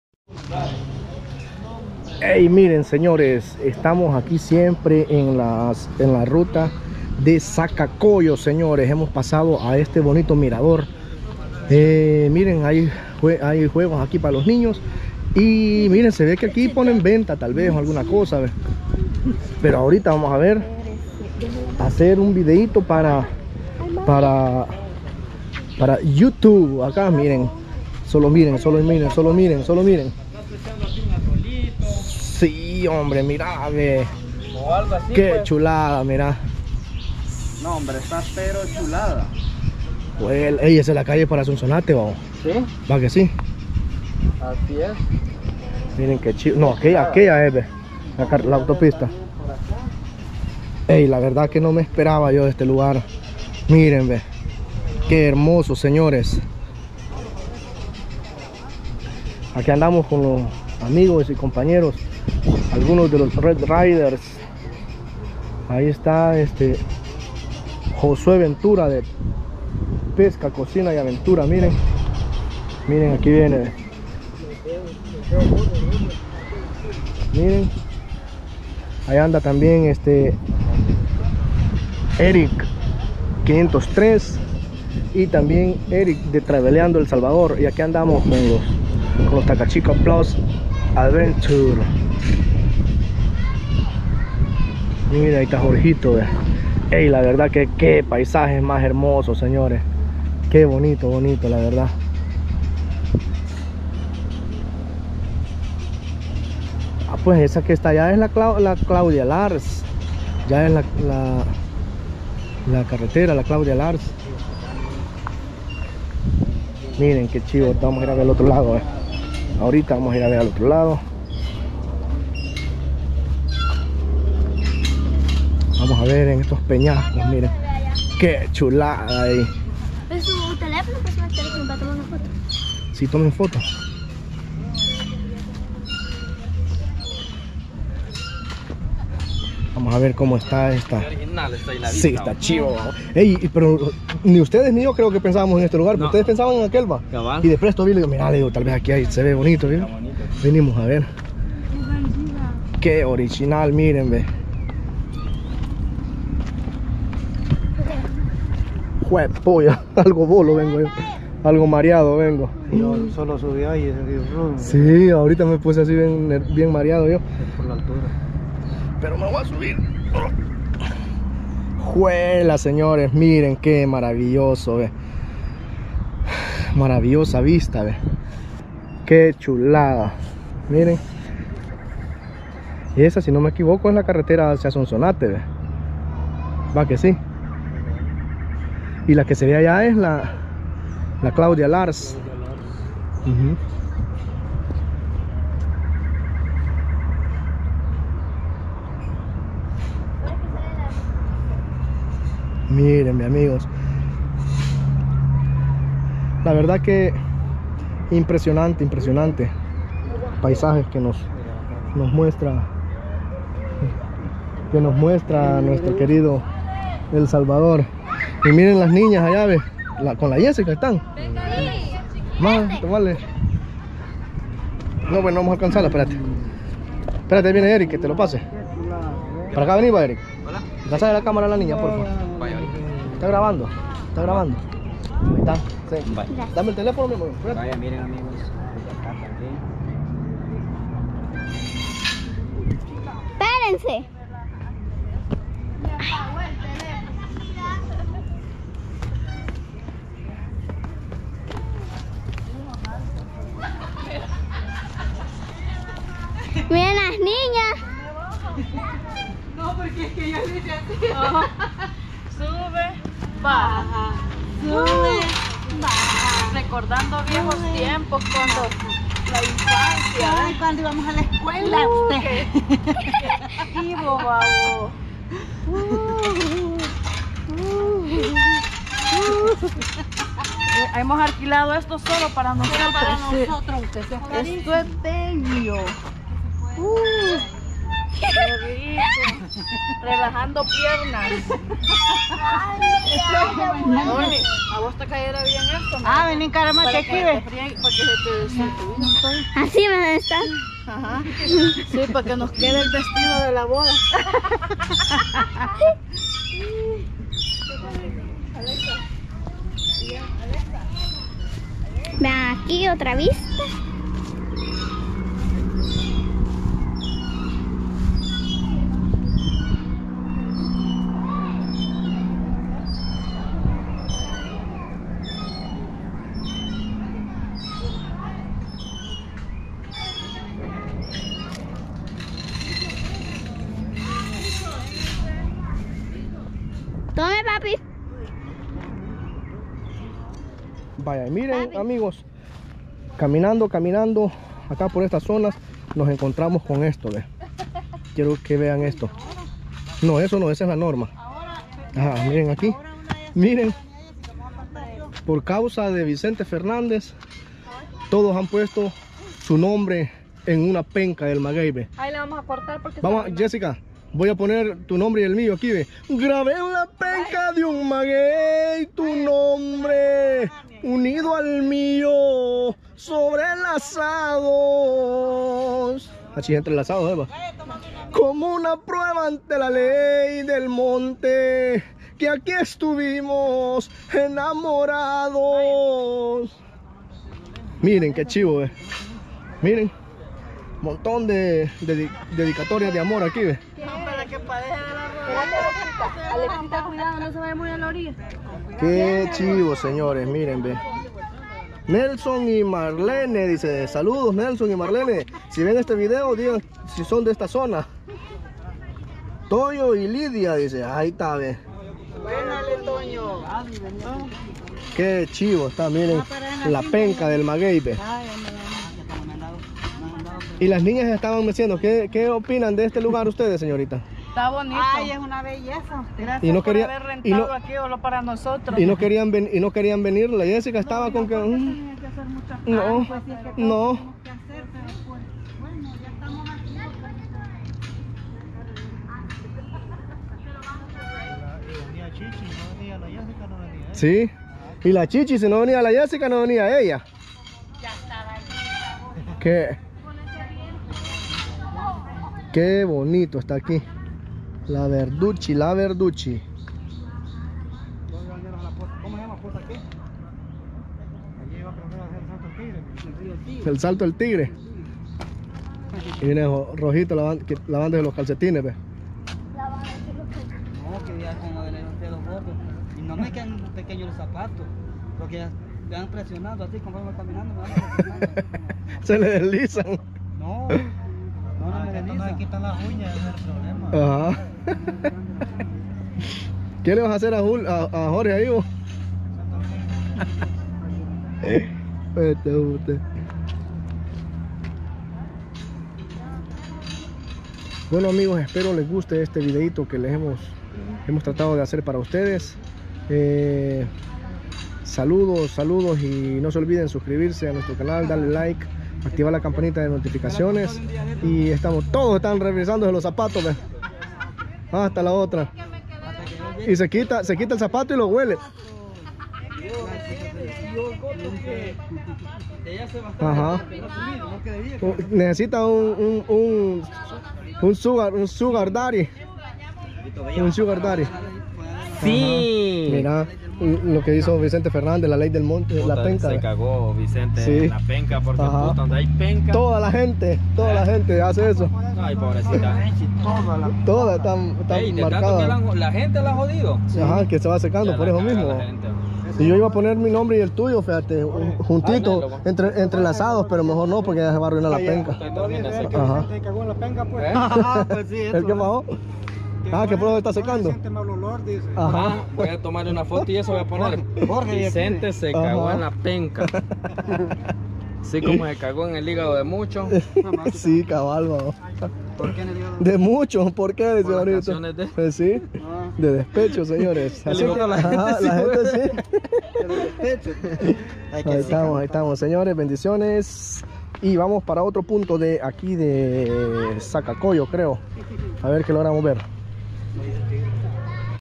ESPECTACULO NATURAL EN EL SALVADOR
Conocimos este maravilloso mirador en sacacoyo